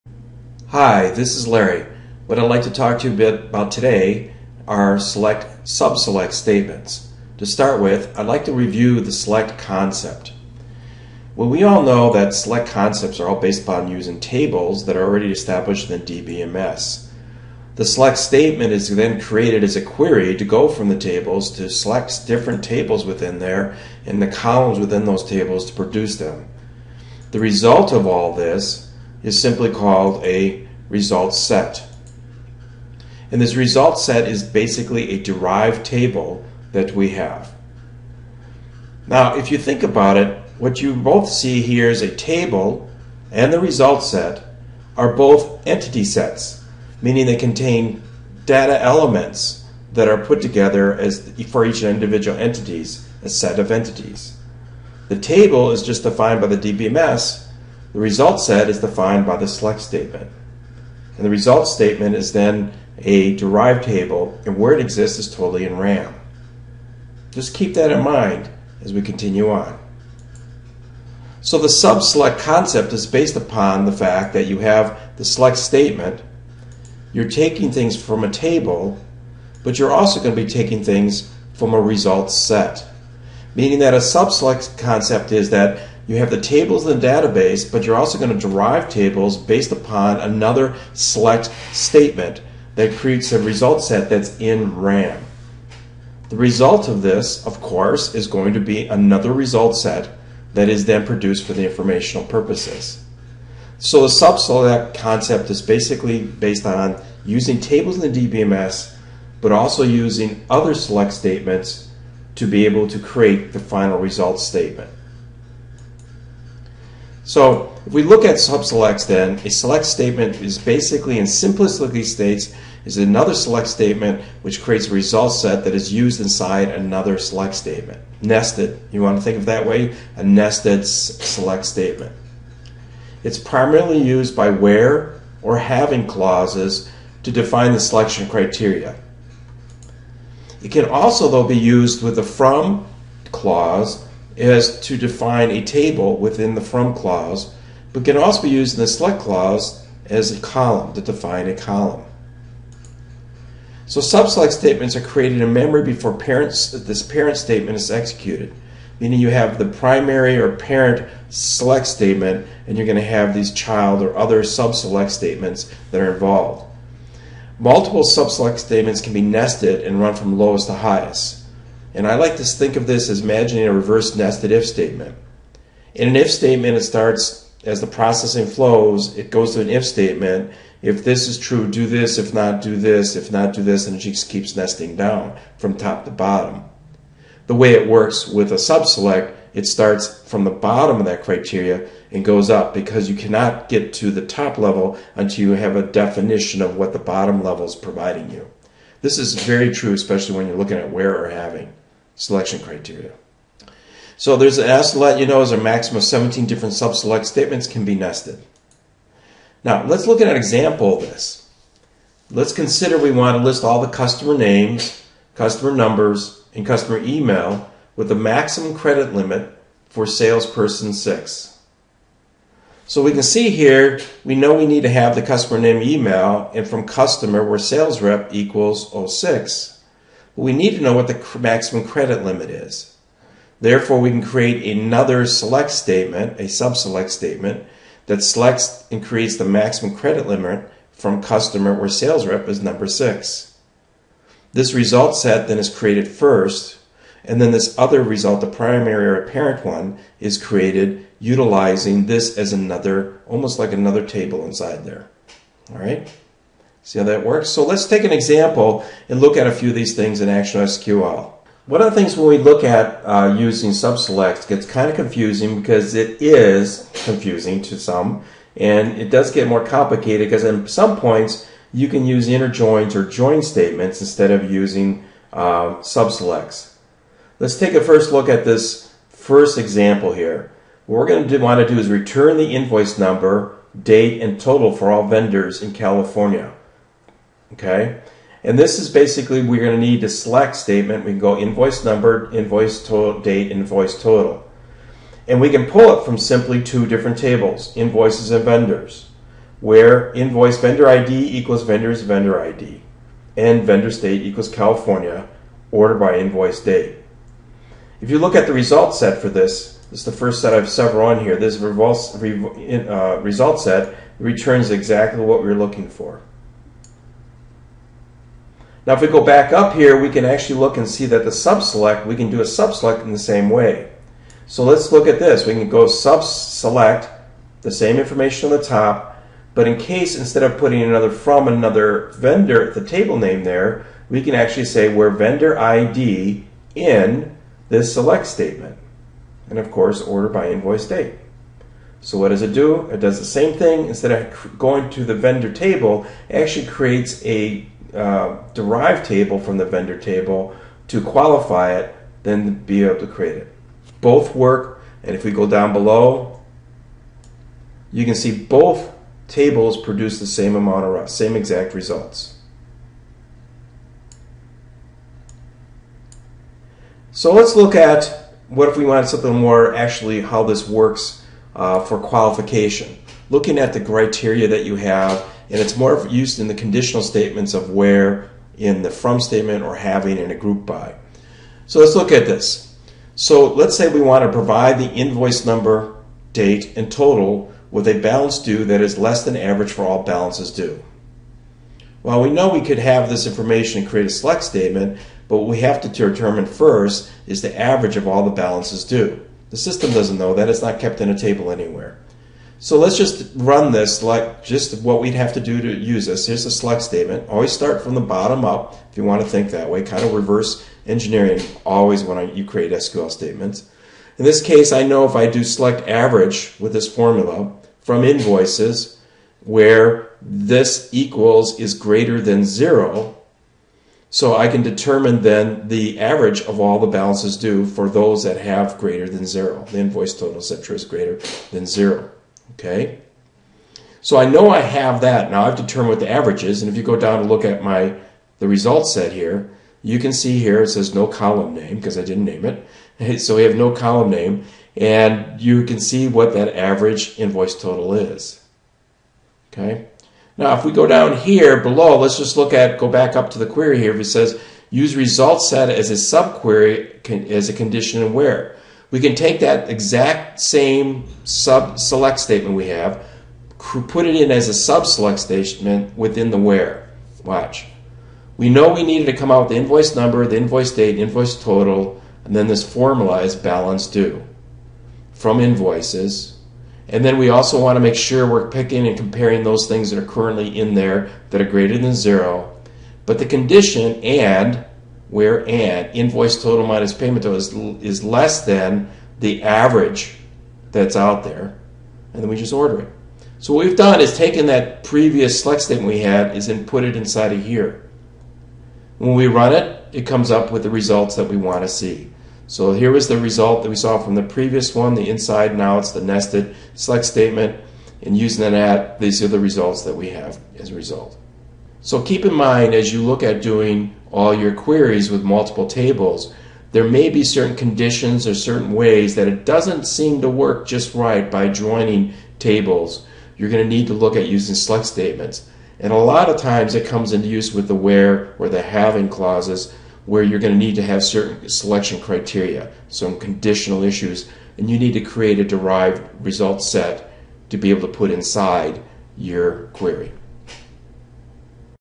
SQL Sub Select Statements
An introduction to the concept and use of Sub SELECT statements in SQL (Structured Query Language).